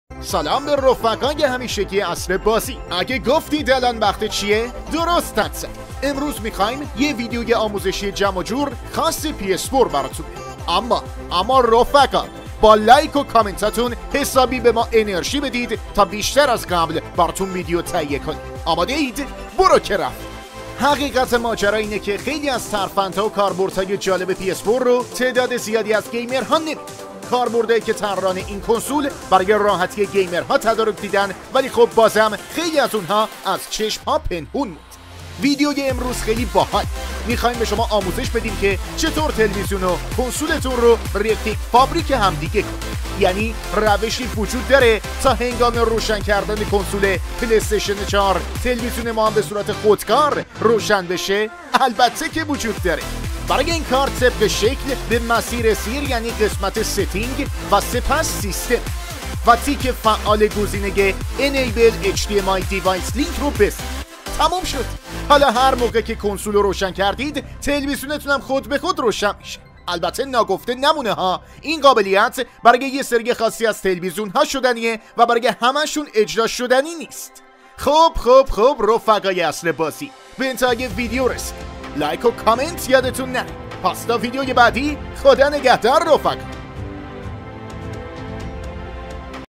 آموزش | روشن کردن اتومات تلویزیون قبل بازی
آموزش | روشن کردن اتومات تلوزیون قبل بازی

با استفاده از ترفندی که امروز به شما آموزش می‌دهیم، می‌توانید هنگام روشن کردن کنسول خود، تلویزیون را به صورت خودکار روشن کنید.
پلی‌استیشن 4 قابلیت‌های گوناگون و البته مخفی دارد که با استفاده از آن‌ها، می‌توانید تجربه‌های جدیدی داشته باشید. آموزشی که امروز قصد داریم در اختیار شما قرار بدهیم، یک ویژگی بسیار جالب است که به عنوان یک ترفند آن را نمایش می‌دهیم.
آیا روشی وجود دارد تا هنگام روشن کردن کنسول پلی استیشن 4، تلویزیون ما به صورت خودکار روشن شود؟ بله! با استفاده از این روش، هنگامی که کنسول پلی‌استیشن 4 را روشن کنید، تلویزیون شما نیز به صورت خودکار روشن خواهد شد.

برای این کار ادامه ویدیو رو ببینید